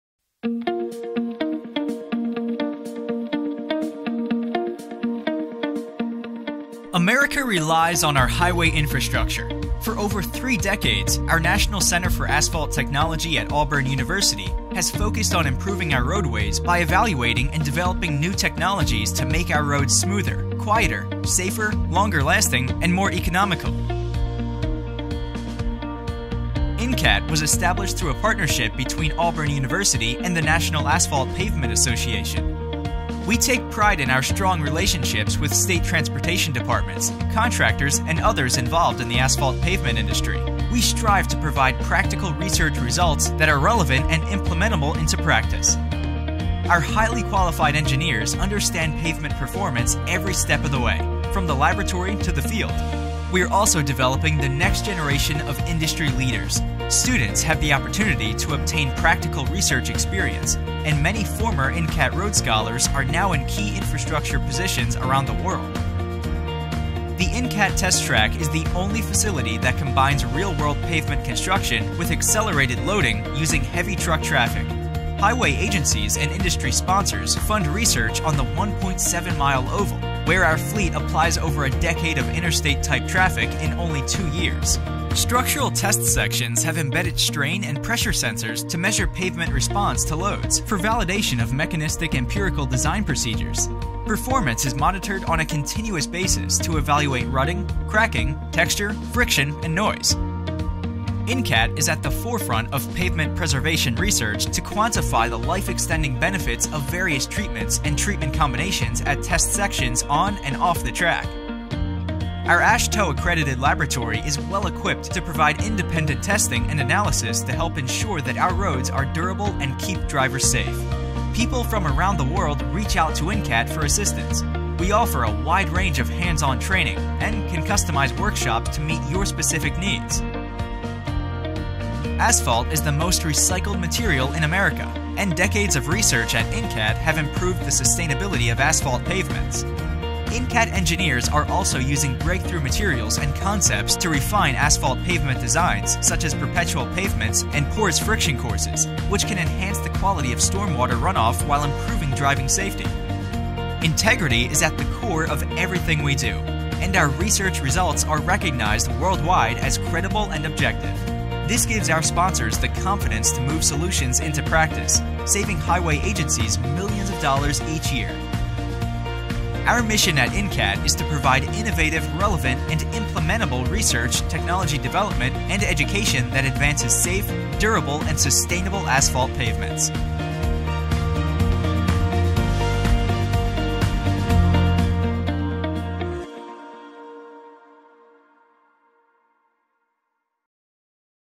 National Center for Asphalt Technology (NCAT) Mission
NCAT’s mission is to provide innovative, relevant and implementable research, technology development and education that advances safe, durable and sustainable asphalt pavements.